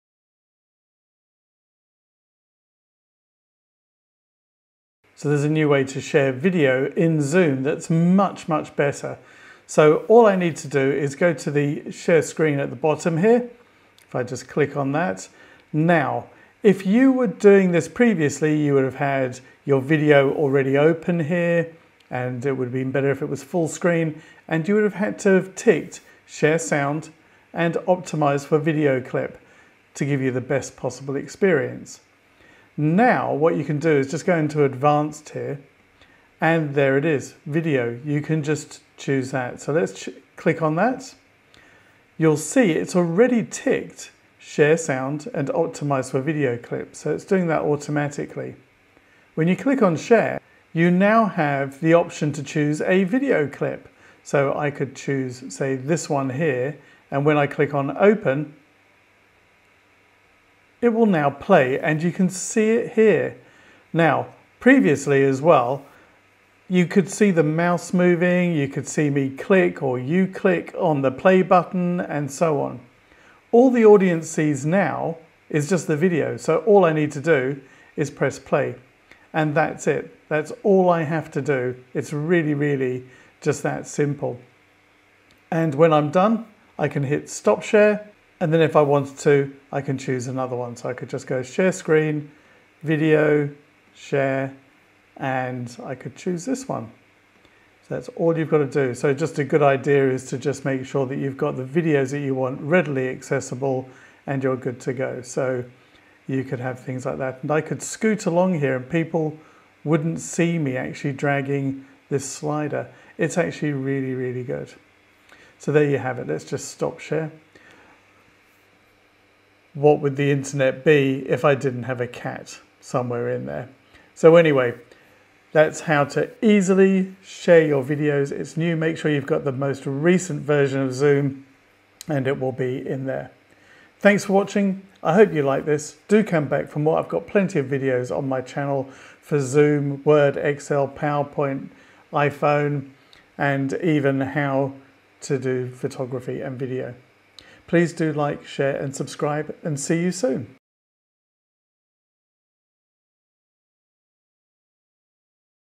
Zoom: The New and Better Way to Share and Play Video in a Meeting or Webinar
Until now you shared a video in Zoom by sharing the screen and choosing it from one of your programs that was open. It was best to play full screen and make sure certain options were selected like optimize for video clip.

Many complained that the video did not play well.

It had the disadvantage too of participants seeing the mouse move and click on the play button or slider to scrub through the video. Not any more. Just make sure you have the latest version on Zoom.

This is for if you have the video file, if you are playing back from somewhere like YouTube or Vimeo then you would choose that site when you share the screen, like you would have done before.

To see how you had to do this here is a video I did on that.